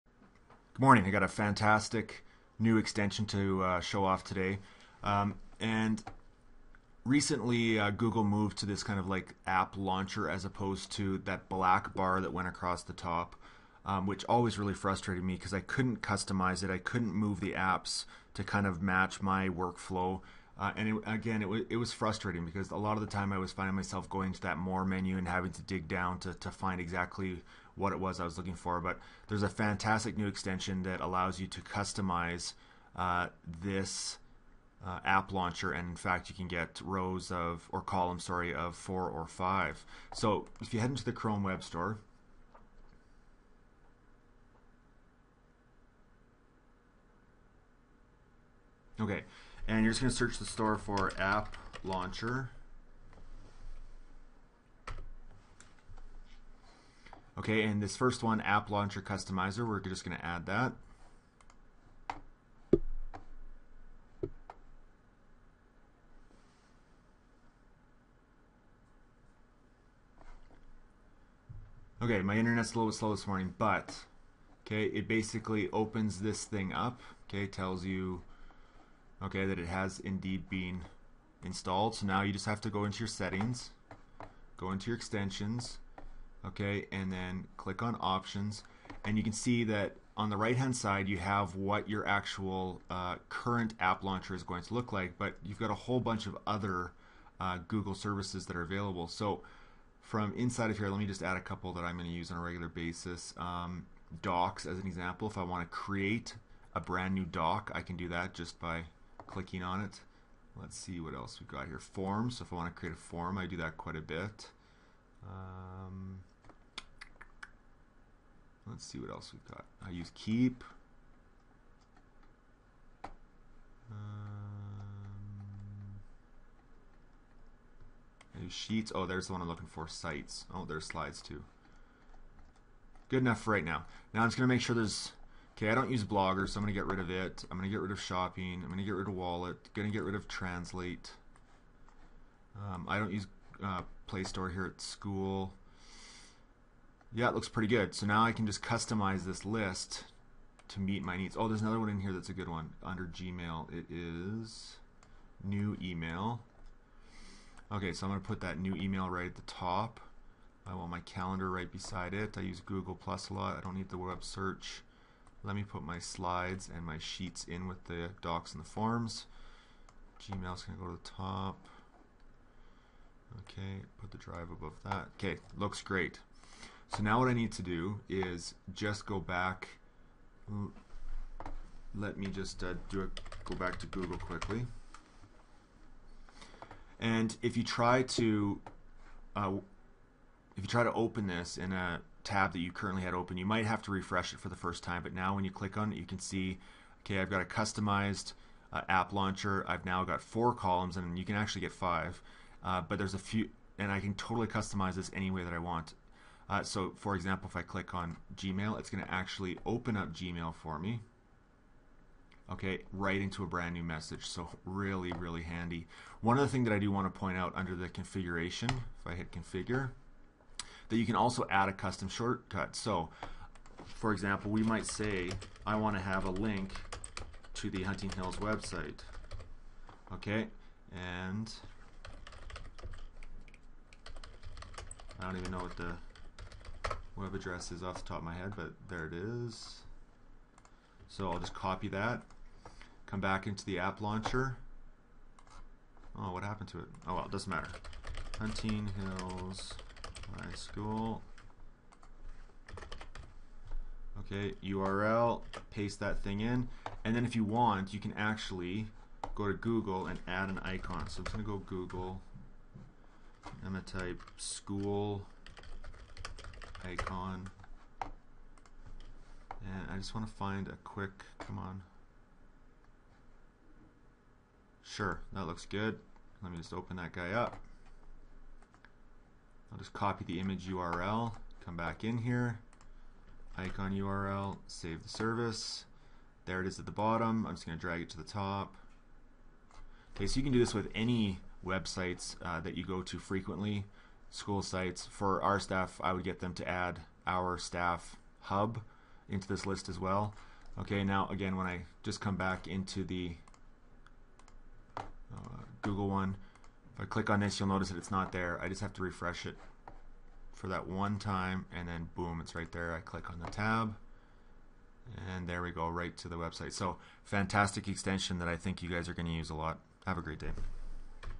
Chrome Extension - App Launcher Customizer
A fantastic extension that allows you to customize your App Launcher.  Holy Recommended Batman.